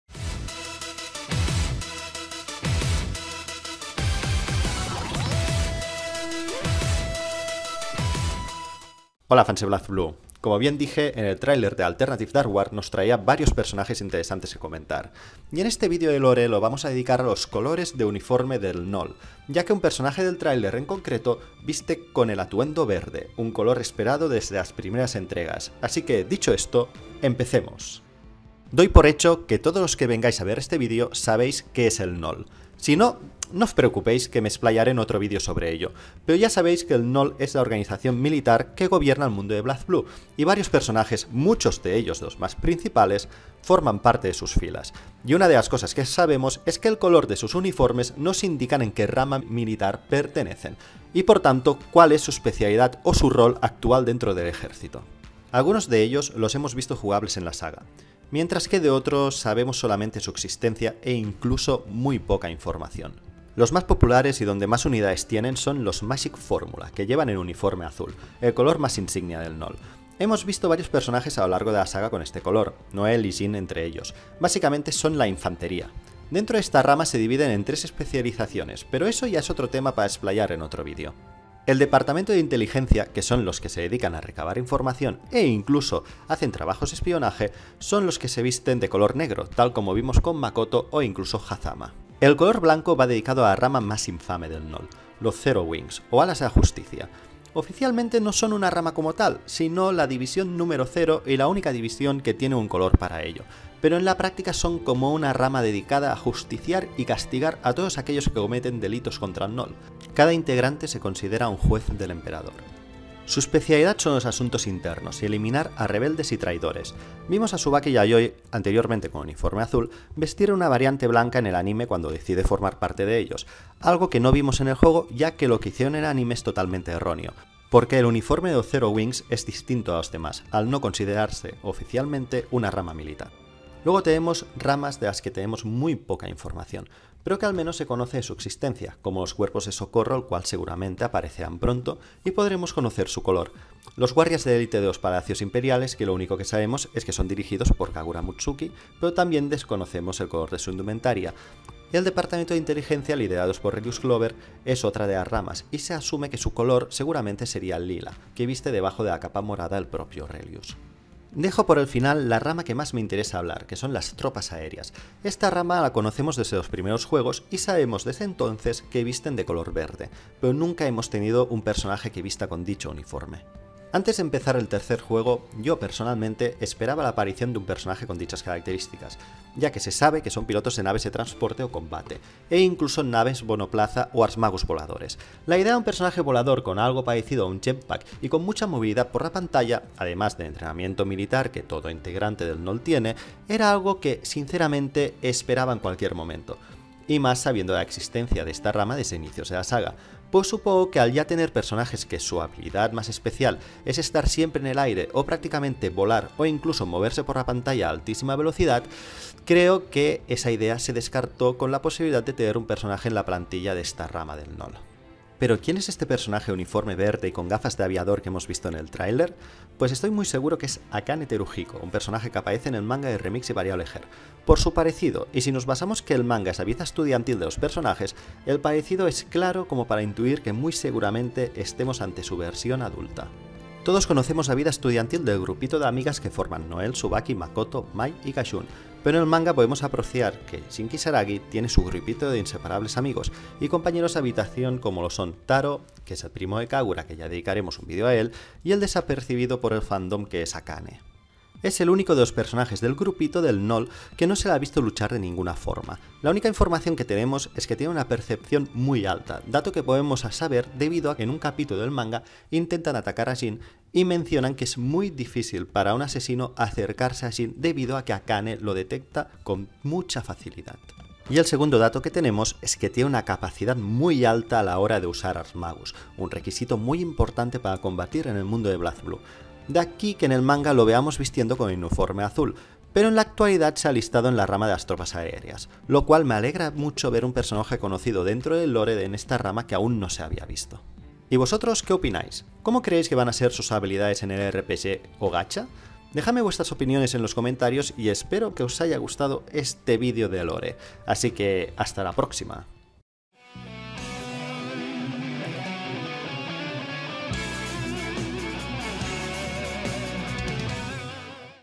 El color de los uniformes en BLAZBLUE. Akane viste uniforme verde en Alternative Dark War.
This video has the option of English subtitles.
Os explico el significado de los colores del NOL, y quien es Akane Teruhiko que hemos visto en el trailer de Alternative Dark War.